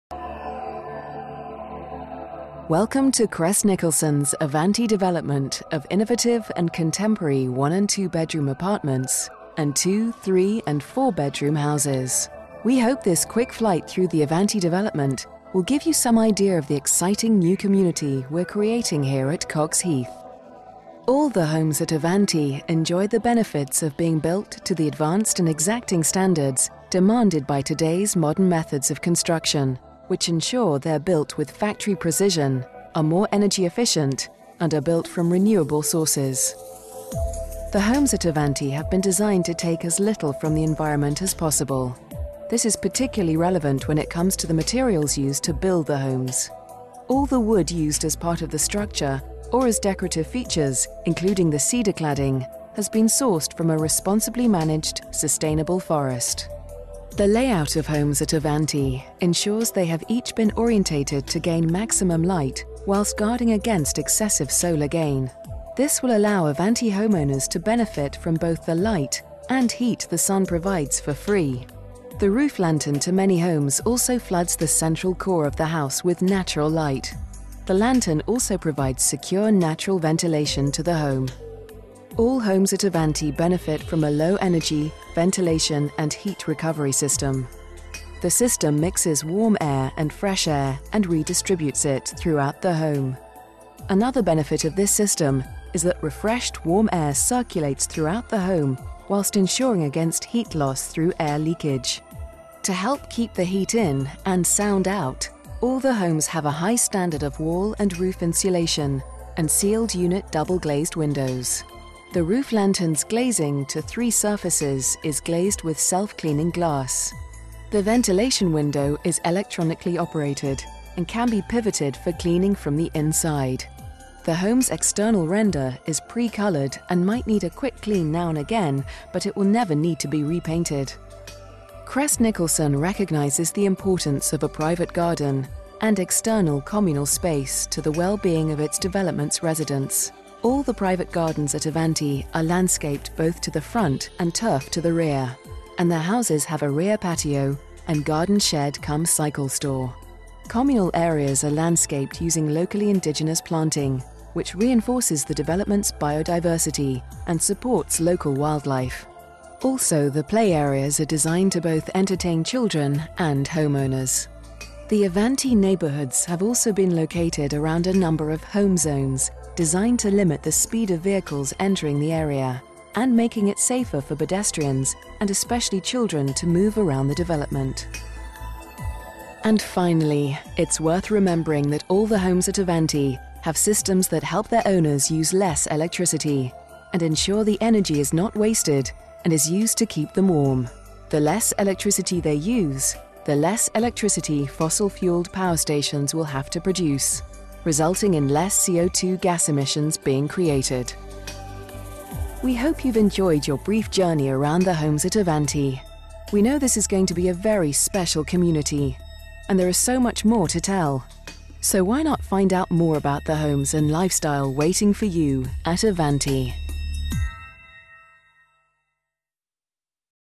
3D Architectural Home House Animation - Avante, Coxheath Maidstone Kent
Full marketing package which included a fly-though animation produced for Crest Nicholson to aid potential clients visualise the Avante residential development in Maidstone, Kent To also see the CGI's view our exteriors tab above by 3D Visuals